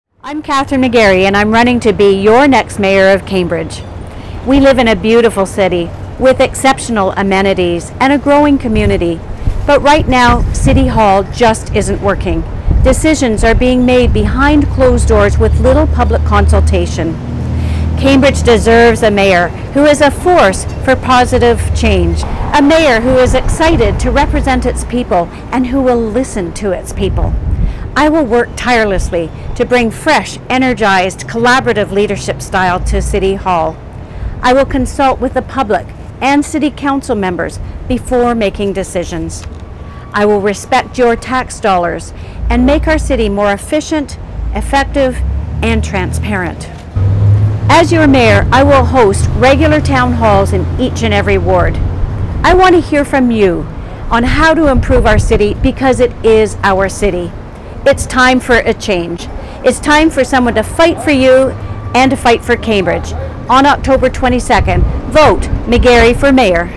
Vote McGarry for Town Hall meetings across Cambridge
We live in a beautiful city, with exceptional amenities and a growing community… but right now, city hall just isn’t working.
Decisions are being made behind closed doors with no public consultation.
Cambridge deserves a mayor who is a force for positive change.
A mayor who is excited to represent its people and who will listen to its people.
I will work tirelessly to bring a fresh, energized and collaborative leadership style to city hall.
I will consult with the public and council members before making decisions.
I will respect your tax dollars and make our city more efficient, effective and transparent.
As your Mayor, I will host regular town halls in each and every ward.
I want to hear from you on how we can improve our city, because it is OUR city.
It’s time for a change.
It’s time for someone to fight for you and to fight for Cambridge.
On October 22, vote for me, Kathryn McGarry to be that someone.